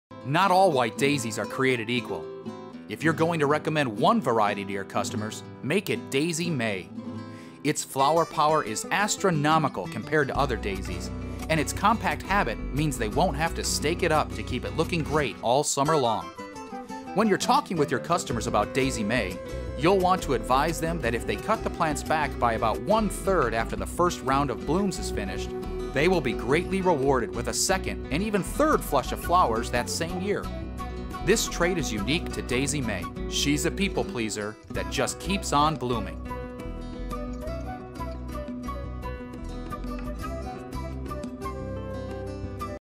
Proven Winners® Gardener Channel: Proven Winners® Daisy May® Leucanthemum
This plant is the gold standard in white shasta daisies, with beautiful 3" white flowers that form a solid dome of blooms. Daisy May is a compact, easy-to-grow perennial with a long bloom time. Deadheading results in multiple rounds of rebloom.

Learn more about Proven Winners and how a better garden starts with a better plant